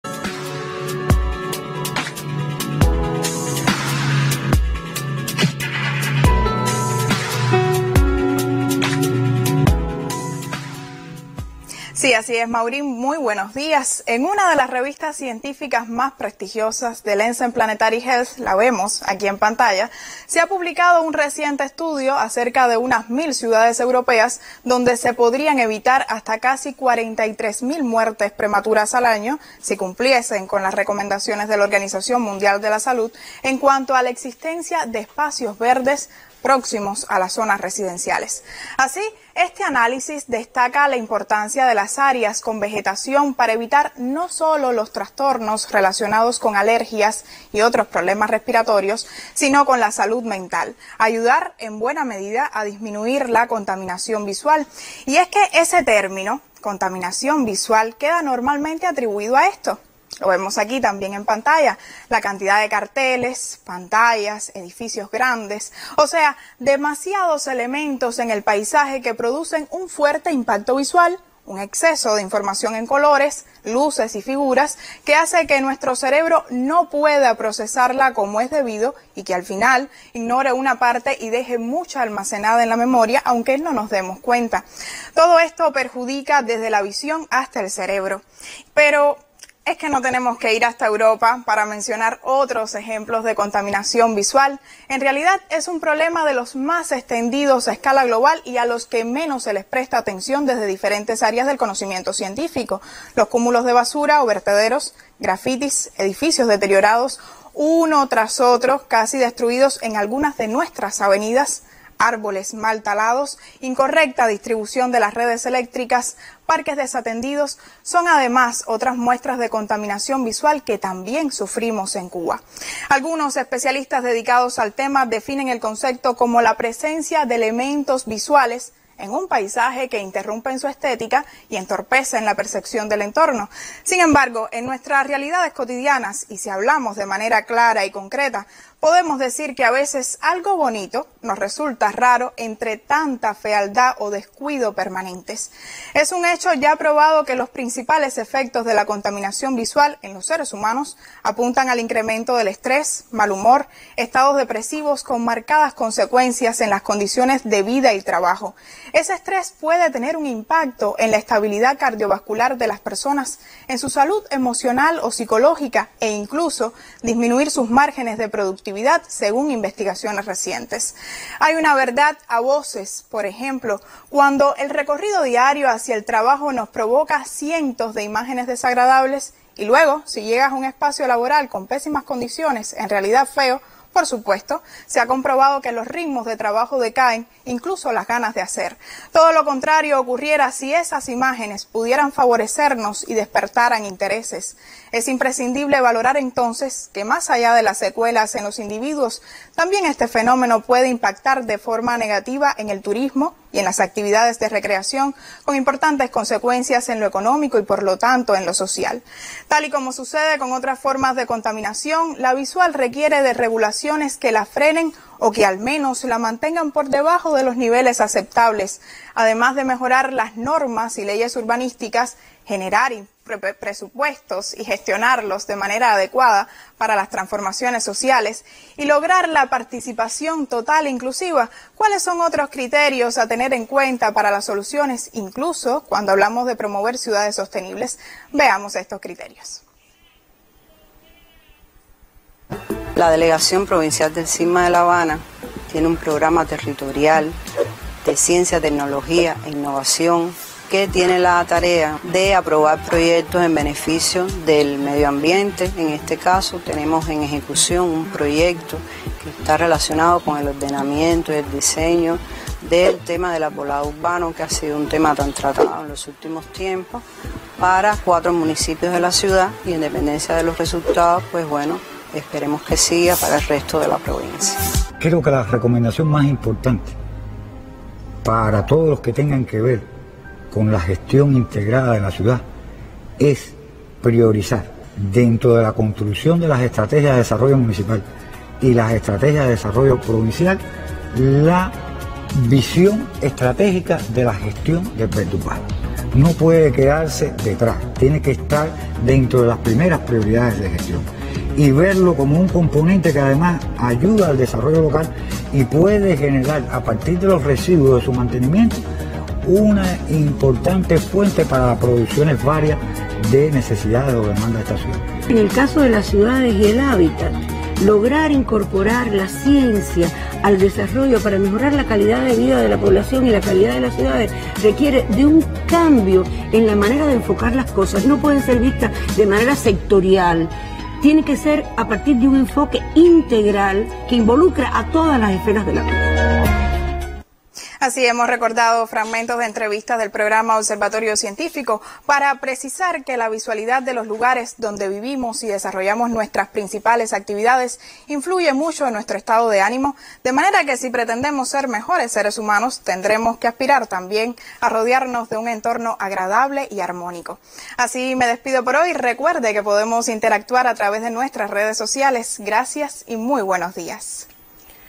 Contaminación visual en Cuba: ¿por qué evitarla?
La basura o vertederos, grafitis, edificios deteriorados, árboles mal talados, incorrecta distribución de las redes eléctricas, parques desatendidos son muestras de ContaminaciónVisual que sufrimos en Cuba. ¿Cuáles son las consecuencias de este fenómeno para la Salud y para el desarrollo del país? ¿Por qué evitarla? La periodista Krystel Aspillaga ofrece un análisis.